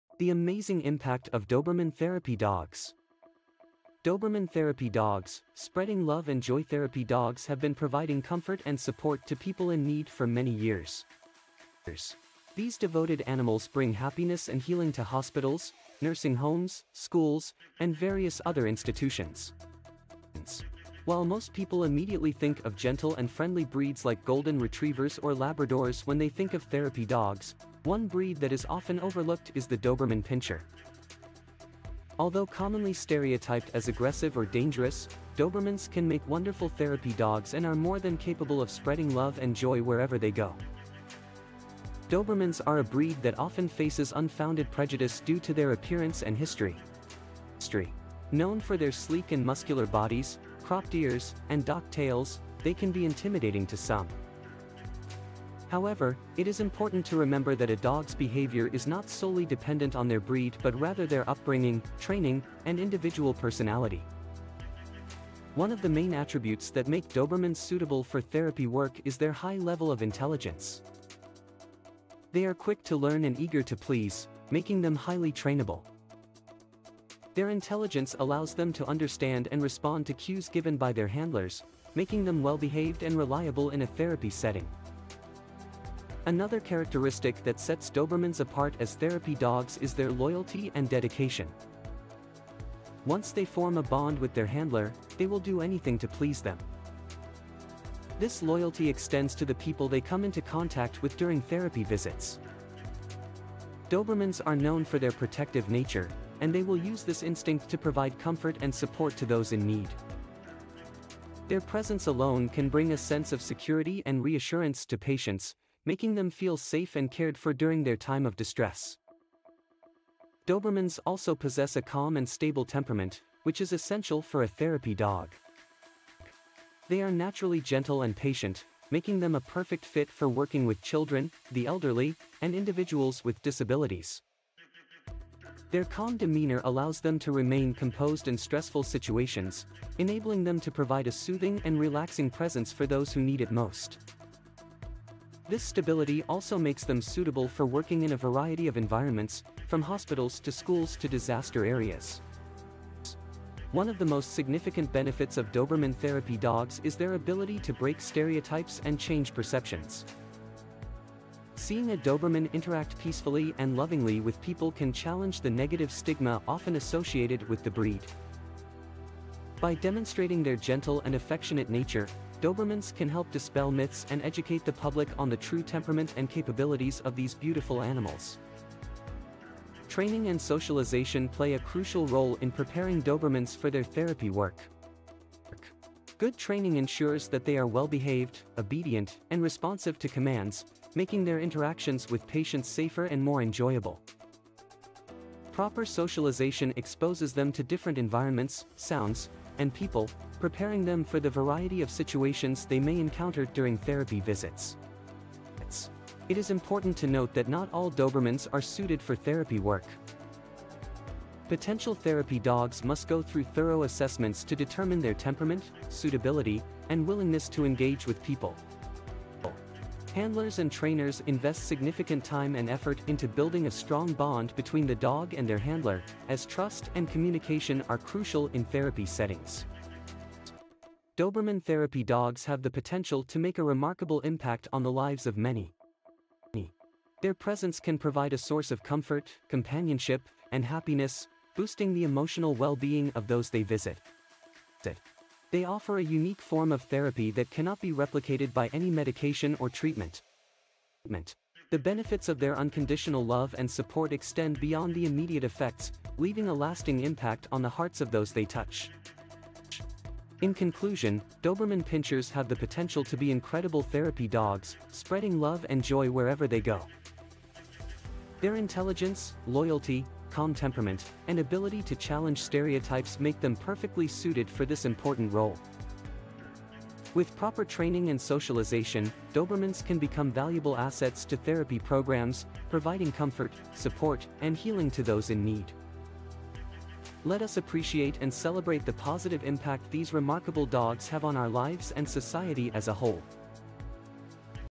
The Amazing Impact of Doberman Therapy Dogs
Doberman Therapy Dogs: Spreading Love and Joy

Therapy dogs have been providing comfort and support to people in need for many years  These devoted animals bring happiness and healing to hospitals, nursing homes, schools, and various other institutions  While most people immediately think of gentle and friendly breeds like golden retrievers or Labradors when they think of therapy dogs, one breed that is often overlooked is the Doberman Pinscher  Although commonly stereotyped as aggressive or dangerous, Dobermans can make wonderful therapy dogs and are more than capable of spreading love and joy wherever they go

Dobermans are a breed that often faces unfounded prejudice due to their appearance and history
00:00 "The Amazing Impact of Doberman Therapy Dogs"